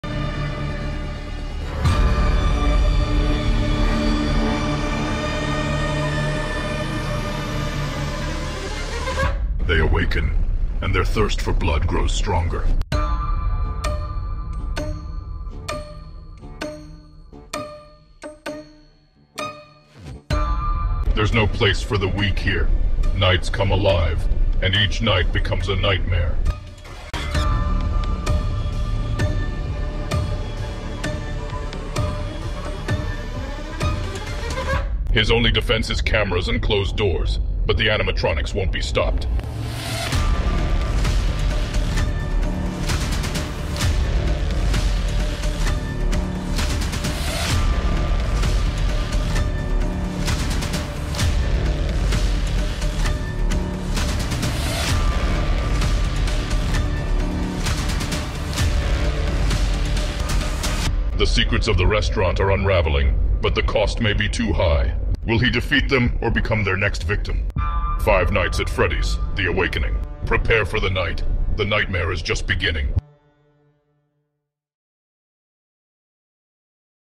Five Nights At Freddy's | AI Trailer
Dare to enter the restaurant where the night becomes your worst nightmare... Introducing FiveNightsAtFreddys AI Trailer. A video fully created by an AI, from the text to the voice with images.